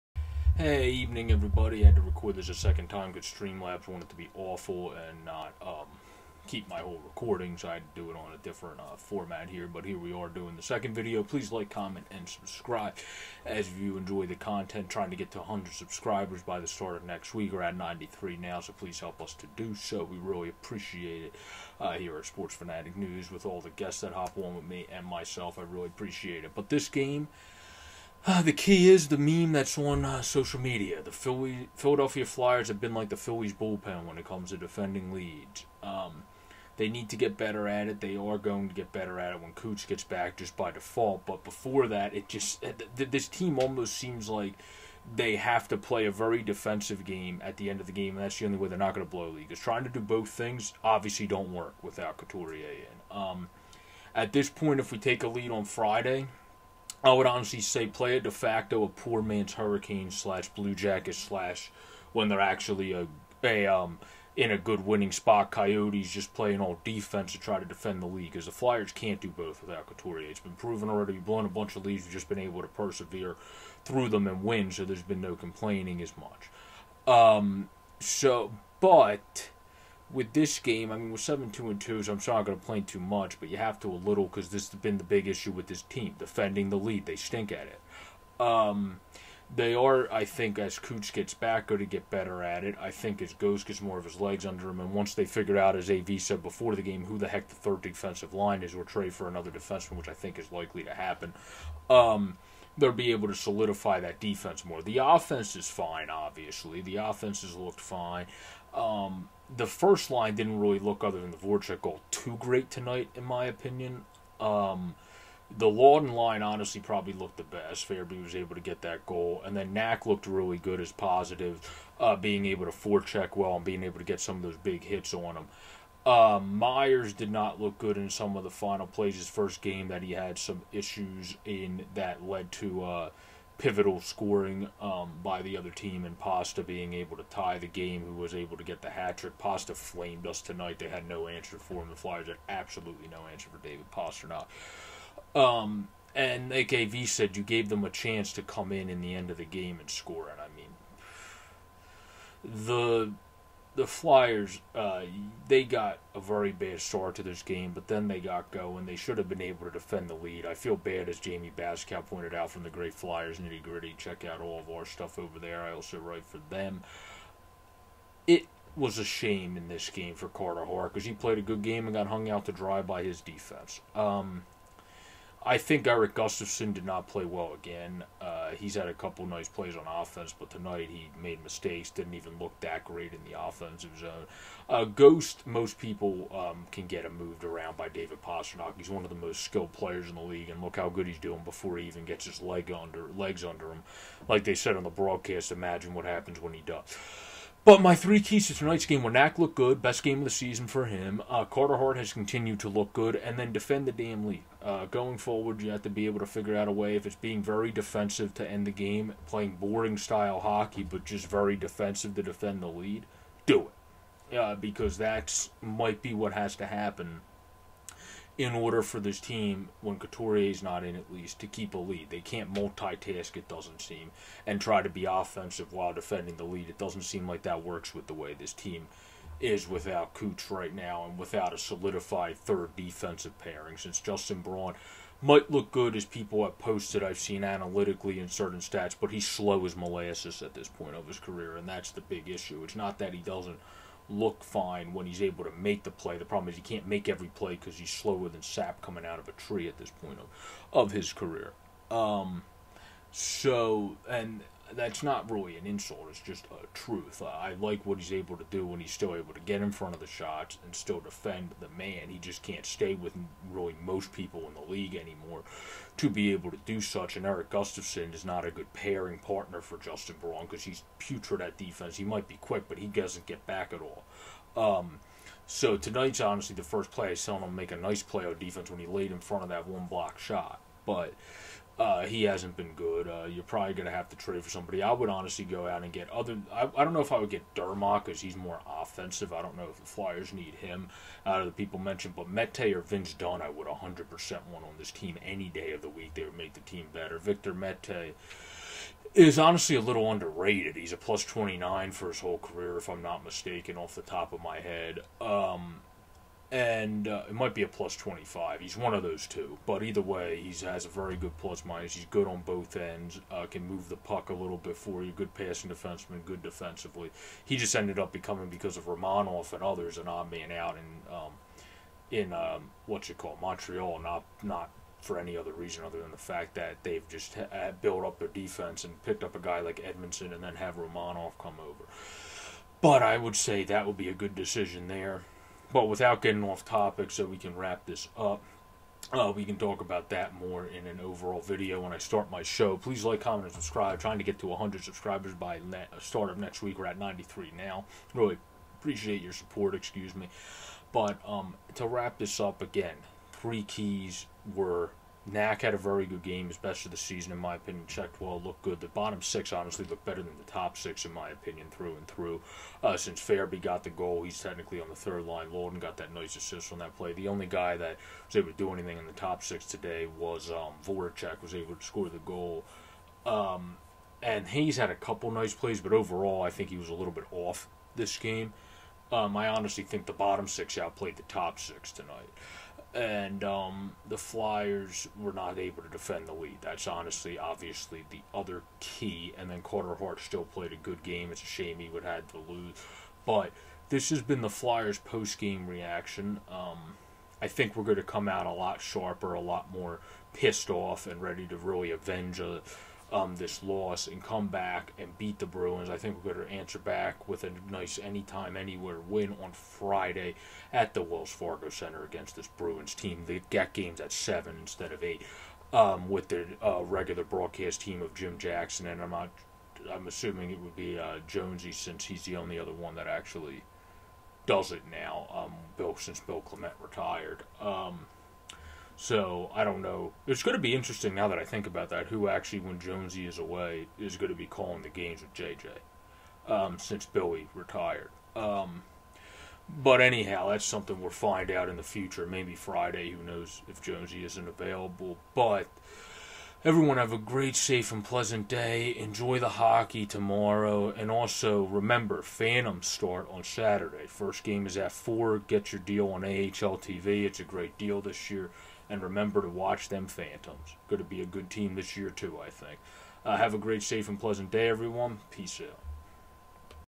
Flyers Brutal 3-1 Blown lead 4-3 OT loss Reaction
Hope you enjoy the video at least more than the game tonight. Awful blown lead time to bounce back for the Flyers on Friday